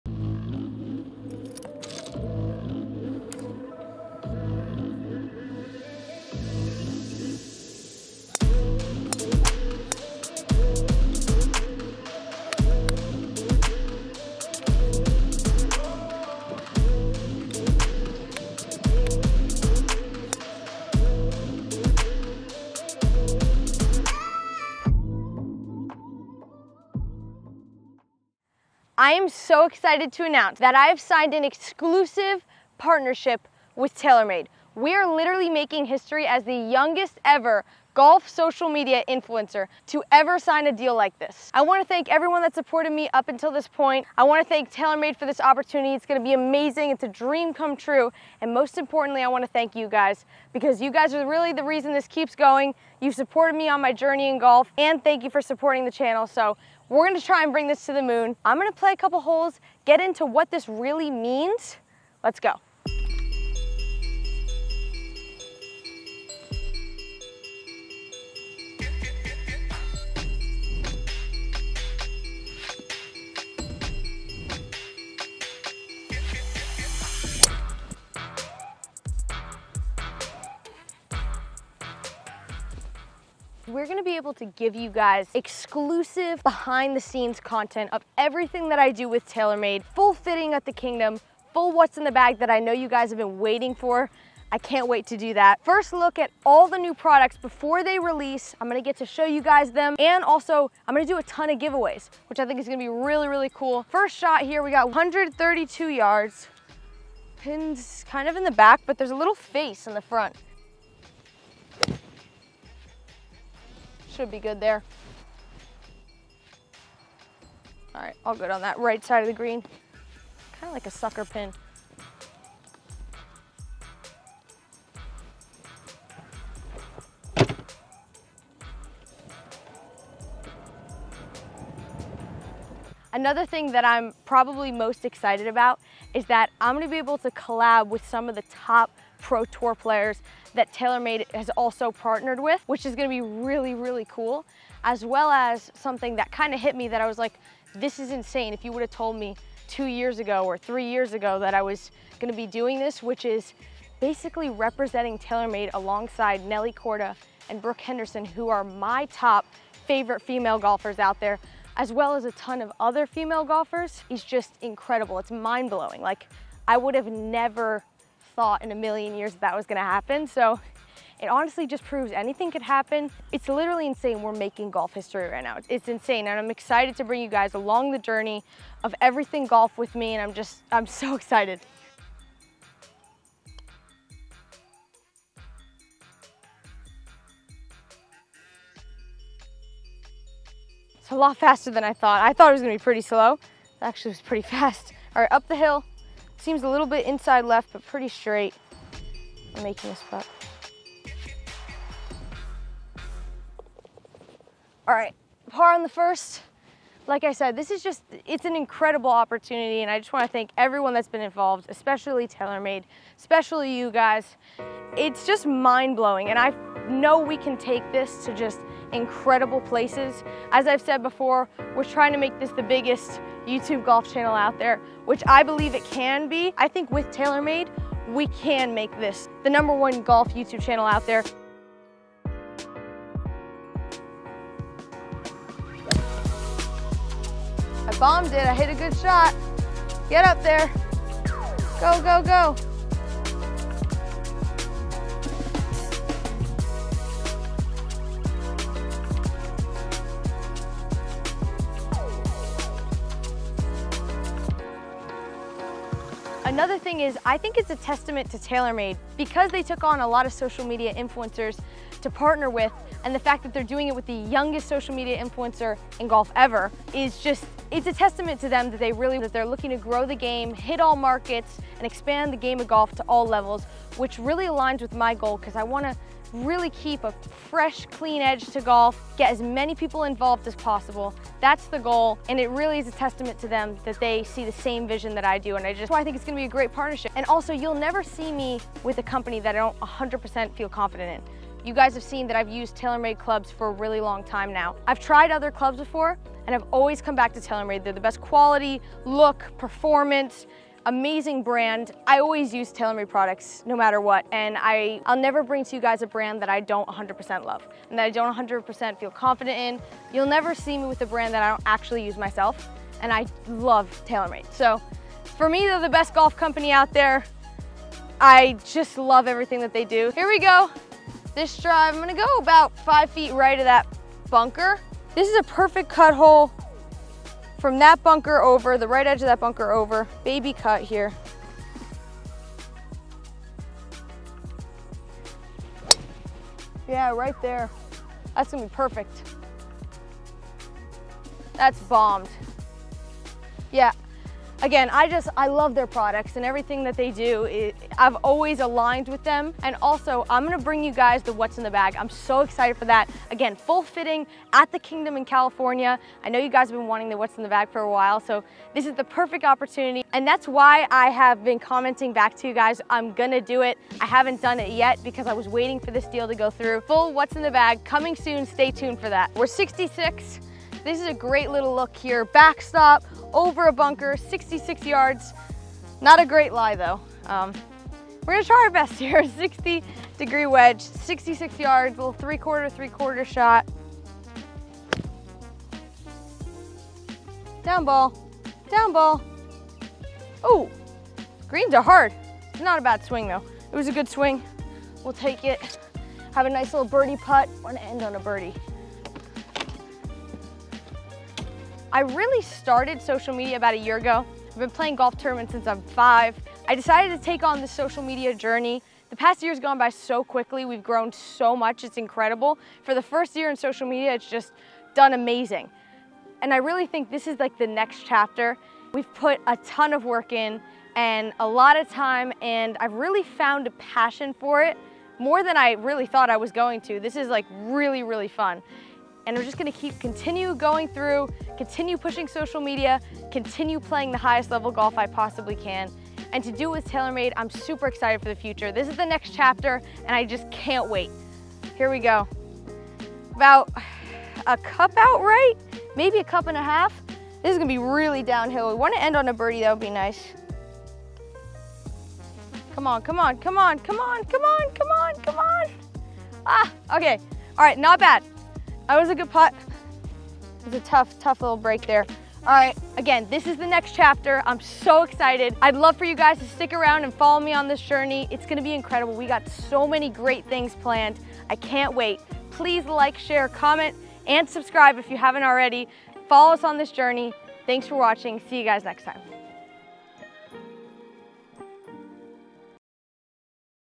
Huge Announcement: MAKING HISTORY
In today's video, Gabby has a HUGE announcement that will have golf enthusiasts buzzing with excitement! Gabby Golf Girl is taking her game to a whole new level as she unveils her newest partner, Taylormade Golf.
But here's the jaw-dropping part: Gabby is making HISTORY by becoming the youngest social media brand ambassador ever to sign a groundbreaking deal like this!
Join Gabby on this incredible journey as she tees off into uncharted territory.
She's breaking boundaries and setting records, and she wants YOU to be a part of it all.
This is just the beginning of a series of incredible things to come, and Gabby can't wait to share them with her amazing community of supporters.